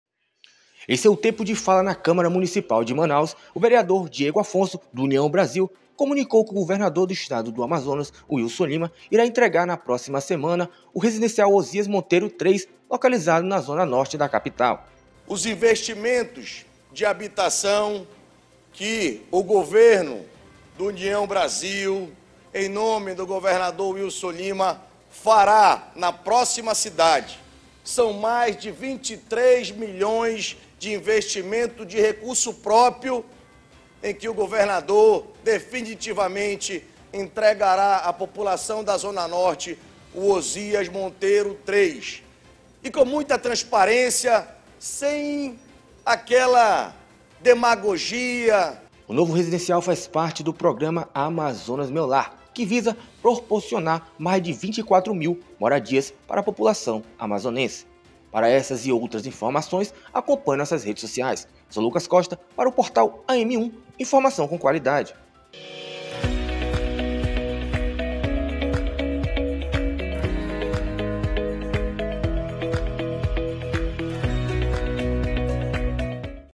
Diego afonso anuncia que governador vai inaugurar Residencial Ozias Monteiro 3 na próxima semana
Wilson Lima vai entregar o residencial Ozias Monteiro três na próxima Semana de acordo com Diego afonso (UB).